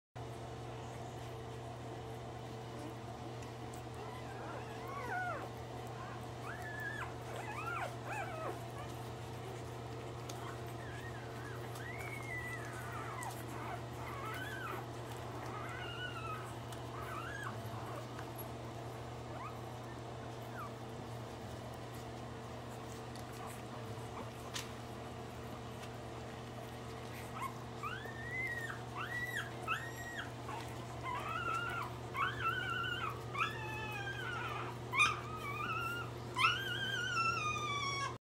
Pack Pitbull Litter, Lucky / Lady 2020 Day 7
Pack pit bull litter born January 9th, 2020, Day 7. Sire: Pack's Lucky, Dame: Pack's Lady. 12 puppies born 9 male and 3 female. Pack of Pitts Kennels Buffalo New York and Niagara Falls Ontario Canada. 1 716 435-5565